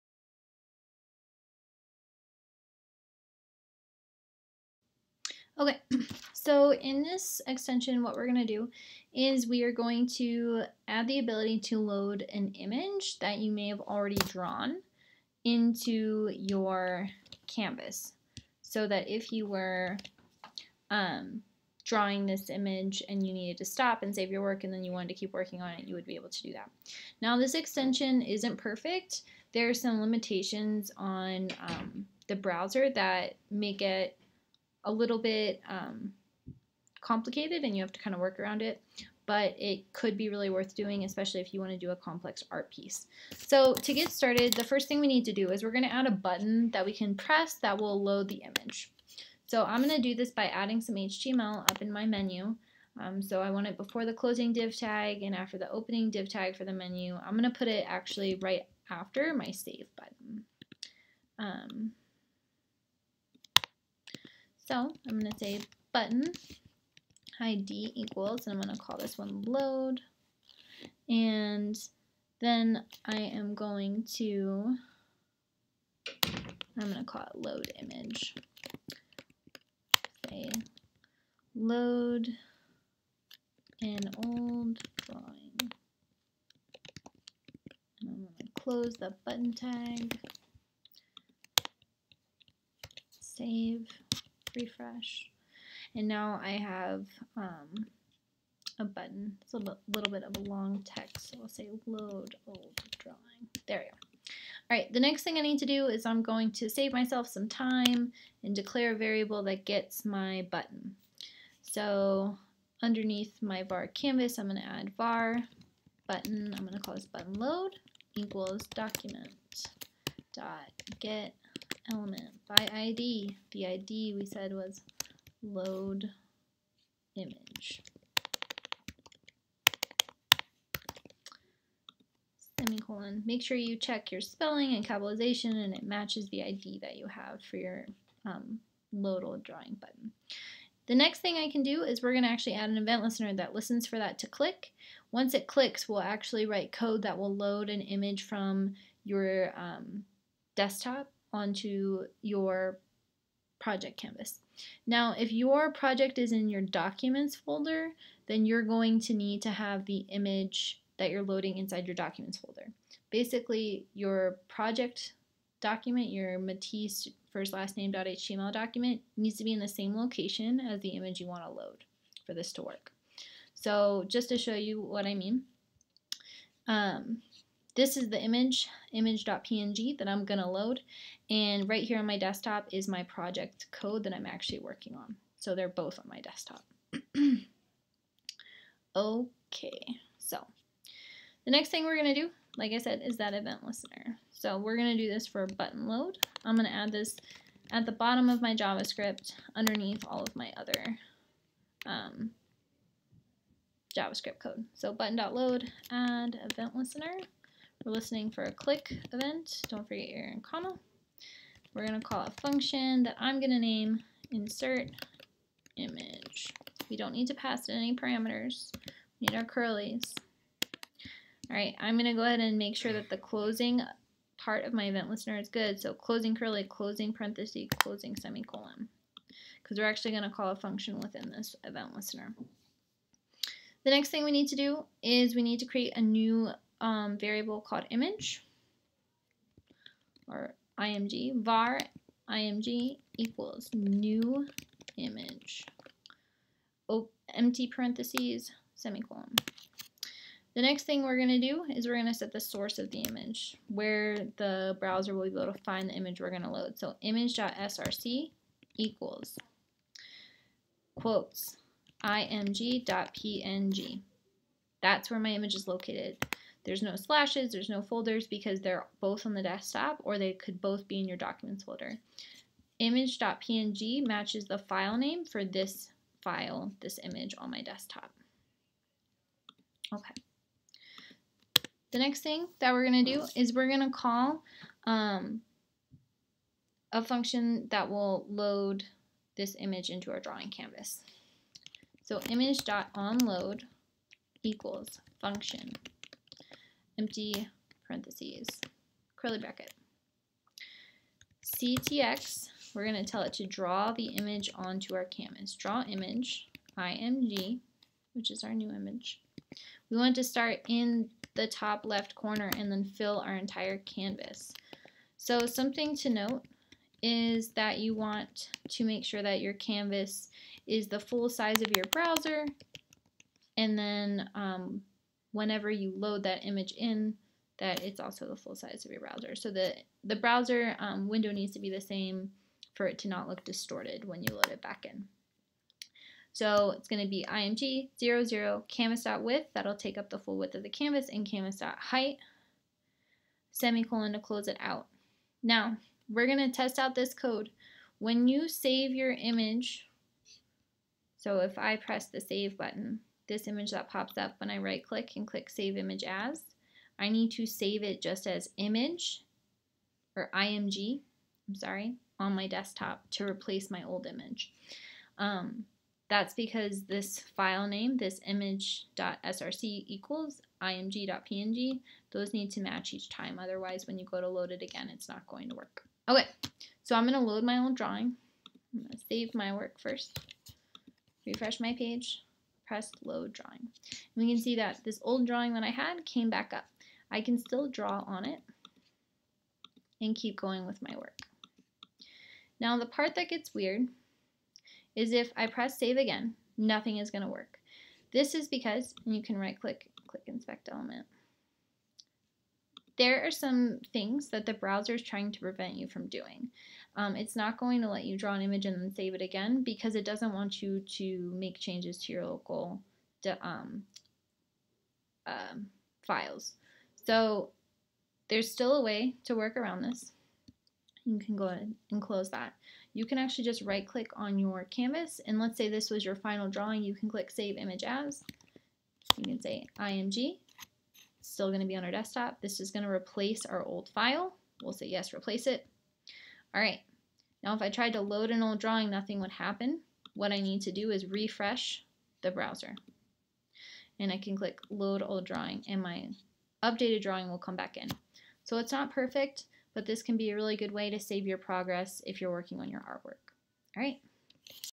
Matisse Project Part 8 - Load Image Extension
In this extension you will learn how to add the ability to load images into your drawing canvas. This is a great way to save your progress of your drawing. You will practice using functions, variables, and event listeners.